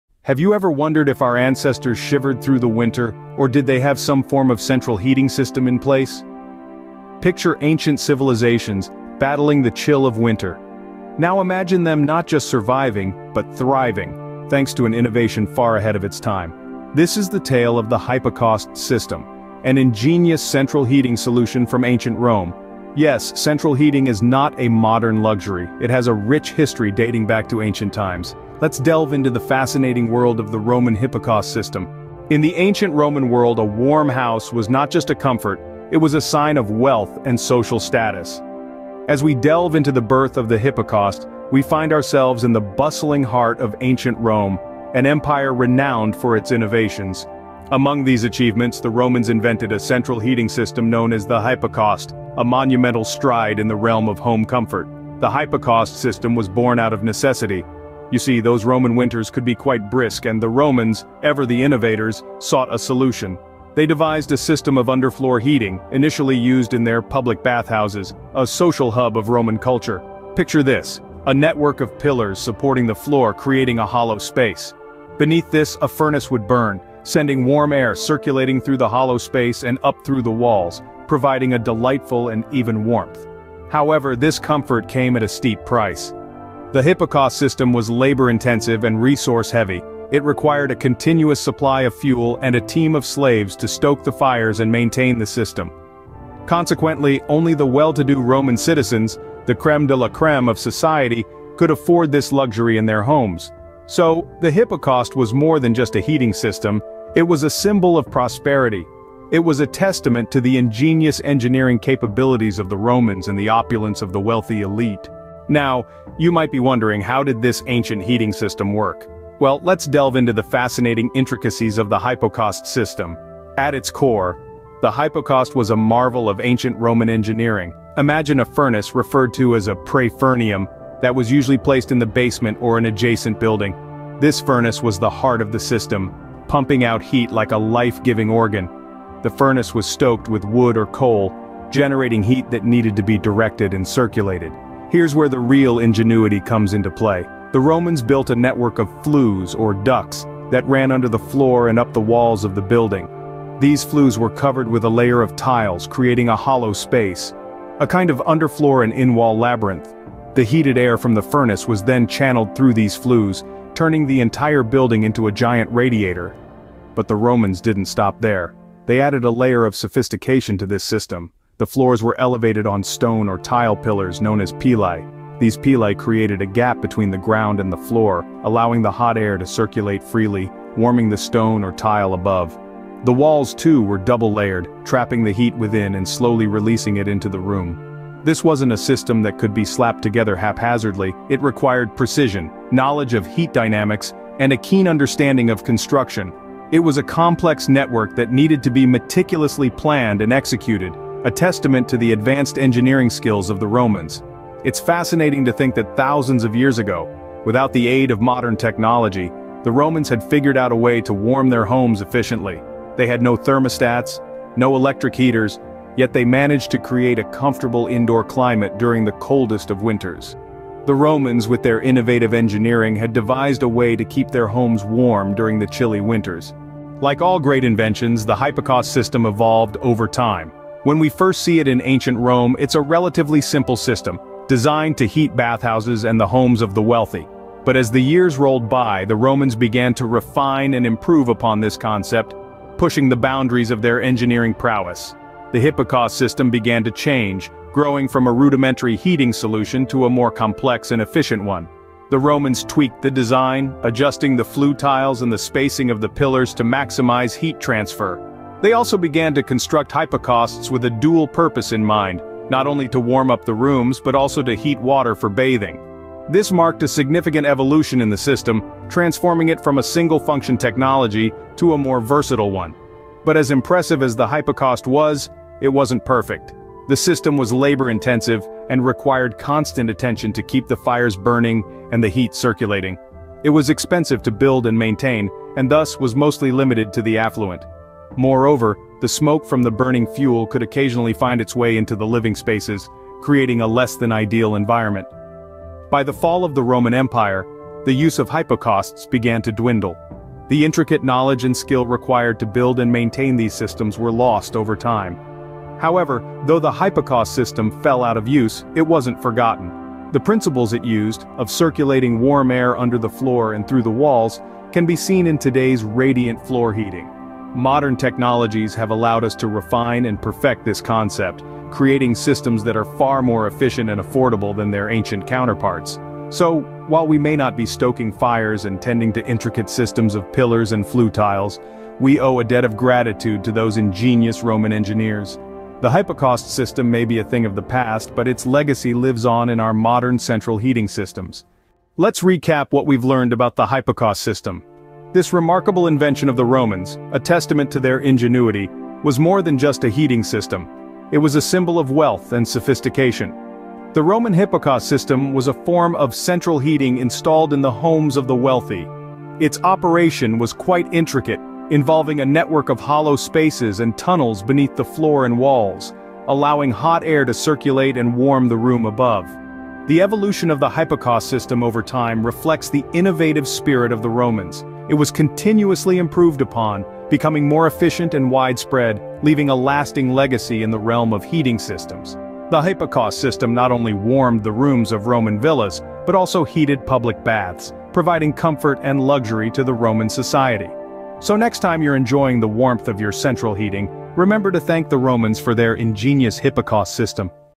Hypocausts - The Ancient Roman way of Central Heating
Step back in time as we explore one of the most ingenious and luxurious features of ancient Roman architecture - the hypocaust. In this fascinating video, we delve into the world of Roman engineering to uncover the secrets of their central heating system.

Discover how wealthy Romans kept their toes toasty during chilly seasons with this advanced technology. We'll take you through the intricate design of hypocausts, showing you how they cleverly circulated warm air beneath floors and through the walls, creating a comfortable living environment that was ahead of its time.

From the grand villas of Pompeii to the lavish bathhouses across the empire, see how this innovative heating system was an emblem of status and comfort. We'll also discuss the engineering and labor that went into building these systems, and how they reflect the broader societal structure of ancient Rome.

Moreover, we'll explore the legacy of Roman hypocausts in modern heating solutions, drawing parallels and noting differences. How did this ancient invention influence our contemporary approaches to heating?

Join us on this historical journey as we unravel the marvels of Roman technology and appreciate the sophistication of an empire that continues to fascinate us centuries later. Get ready for an immersive experience into the warmth of Roman ingenuity with "Under the Floors: The Ancient Roman Central Heating System.